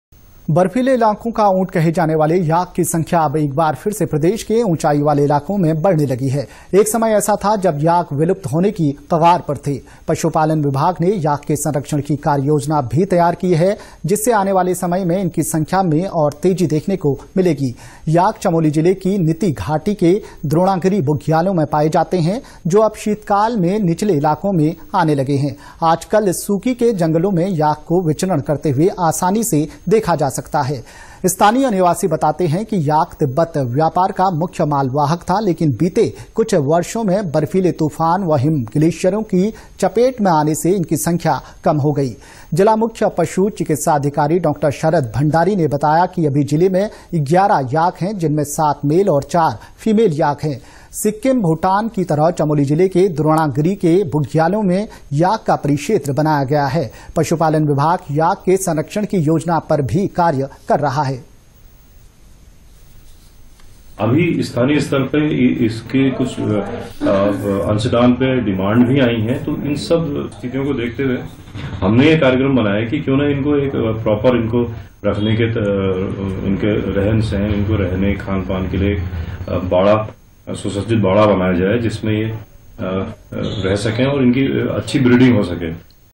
Chamoli : Department of Animal Husbandry aiming to increase Yak population
बर्फीले इलाकों का ऊंट कहे जाने वाले याक की संख्या अब एक बार फिर से प्रदेश के ऊंचाई वाले इलाकों में बढ़ने लगी हैं। एक समय ऐसा था जब याक विलुप्त होने की कगार पर थे। पशु पालन विभाग ने याक के संरक्षण की कार्ययोजना भी तैयार की है। जिससे आने वाले समय में इनकी संख्या में और तेजी देखने को मिलेगी। याक चमोली जिले की नीती घाटी के द्रोणागिरी बुग्यालों में पाये जाते है। जो अब शीतकाल में निचले इलाकों में आने लगे हैं। आज कल सूकी के जंगलों में याक को विचरण करते हुए आसानी से देखा जा सकता है। स्थानीय निवासी बताते हैं कि याक तिब्बत व्यापार का मुख्य माल  वाहक था लेकिन बीते कुछ वर्षो में बर्फीले तूफान व हिम गिलेशियरों की चपेट में आने से इनकी संख्या कम हो गई। जिला मुख्य  पशु चिकित्सा अधिकारी डॉ. शरद भंडारी ने बताया कि अभी जिले में 11 याक हैं। जिनमें 7 मेल और 4 फीमेल याक हैं। सिक्किम-भूटान की तरह चमोली जिले के द्रोणागिरी के बुग्यालों में याक का परिक्षेत्र बनाया गया है। पशुपालन विभाग याक के संरक्षण की योजना पर भी कार्य कर रहा है।